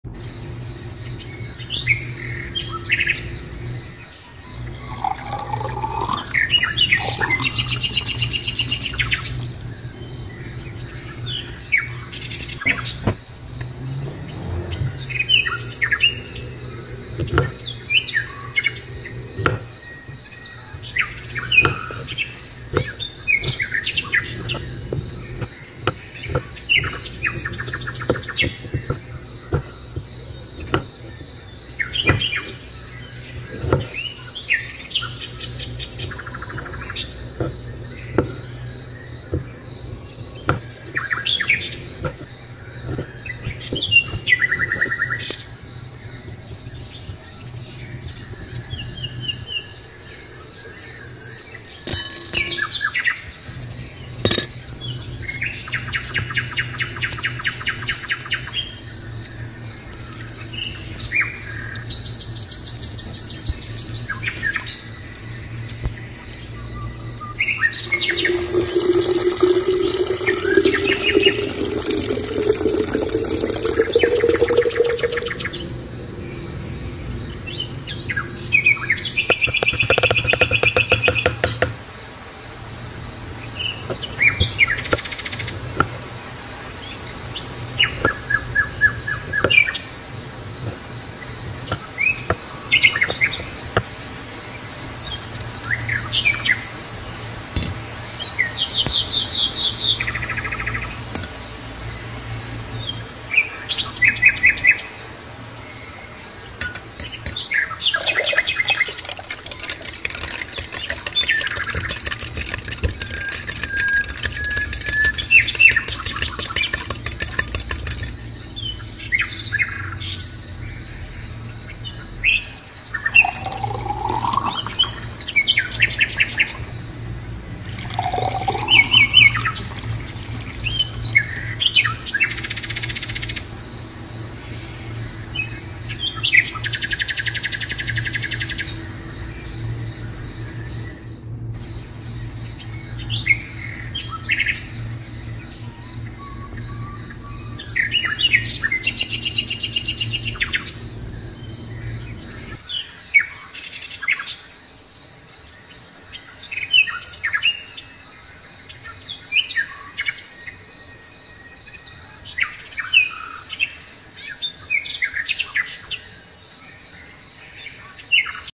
এই গরমে লেবু ও পুদিনা পাতা দিয়ে একগ্লাস লেবুপানি হলে কলিজা ও প্রাণ জুড়িয়ে শরীরে স্বস্তি ফিরে আসবে
পুদিনাপাতা ও লেবু দিয়ে লেবু পানি তৈরি